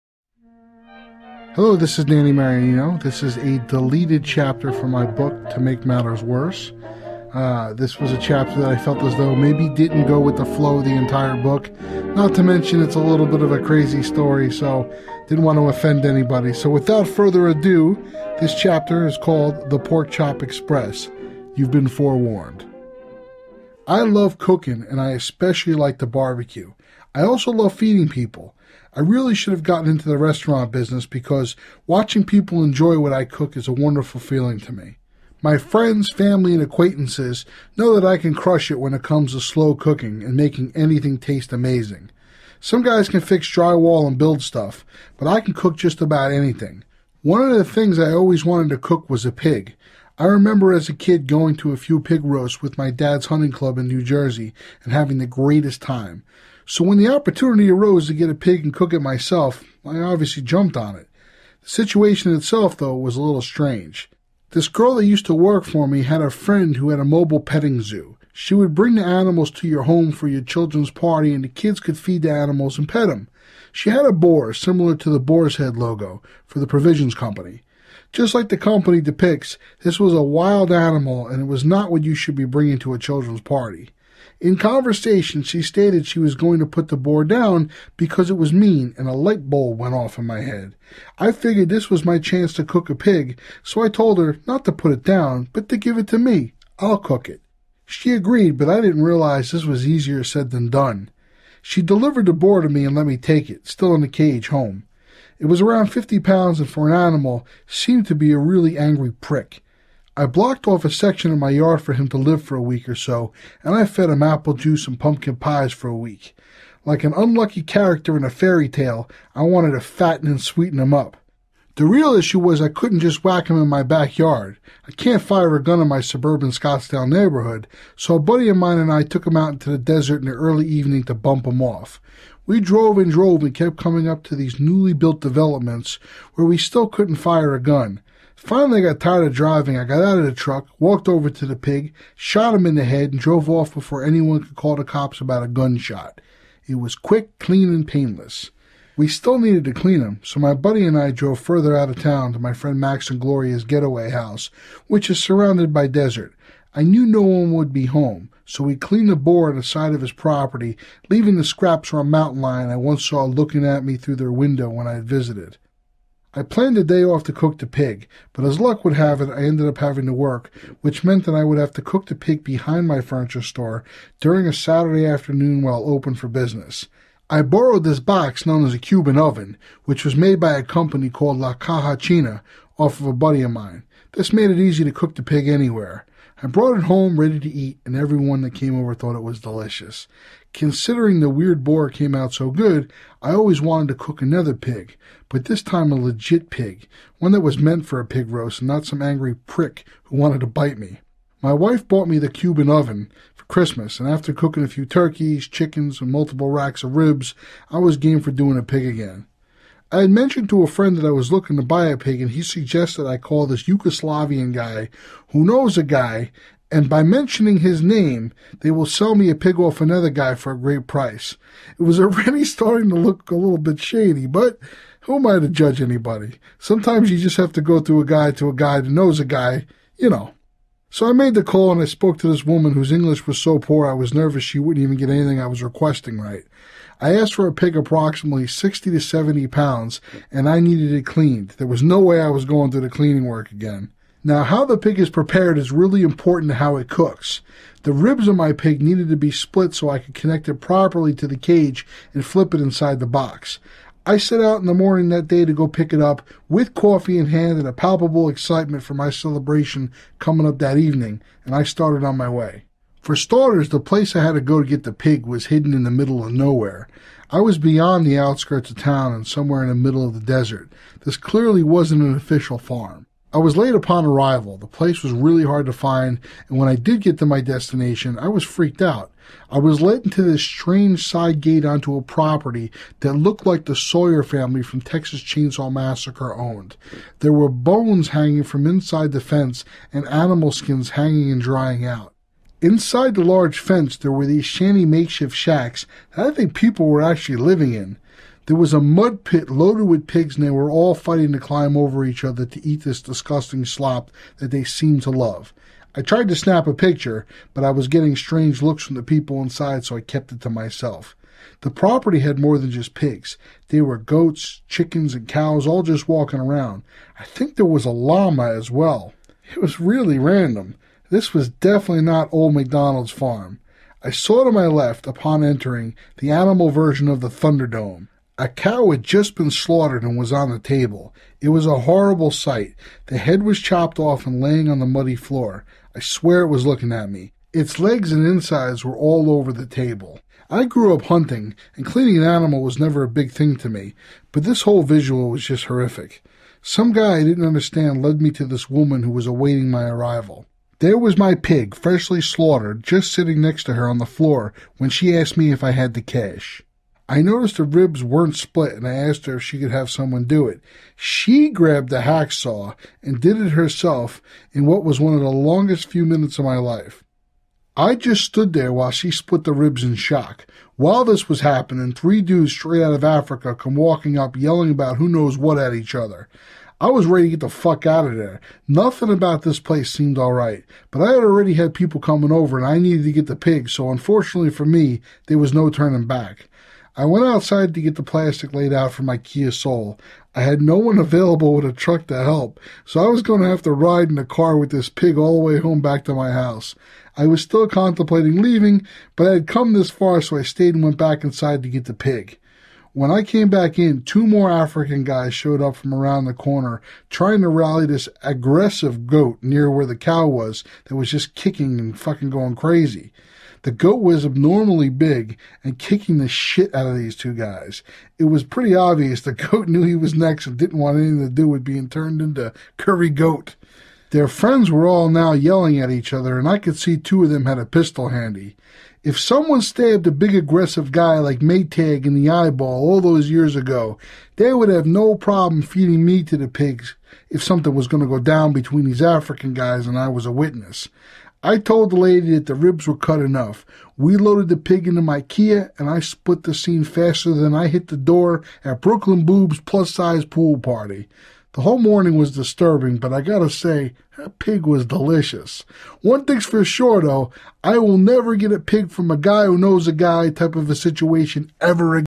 To Make Matters Worse Audiobook by Danny Marianino (Deleted Chapter)
This is a deleted chapter of my new book titled To Make Matters Worse which will be available July 22nd 2016.

The story didn't fit the general theme of the book and I didn't want to upset anyone that may be offended by this crazy quest for the ultimate BBQ.

To Make Matters Worse will be available in print on Amazon and other retailers. The audio book will be available on Audible, ITunes, CD Baby and on CD.

Recorded at Arcane Digital Recording Studio in Mesa AZ - Photo by Eye Photo Photography  - End photo by Matt Sellner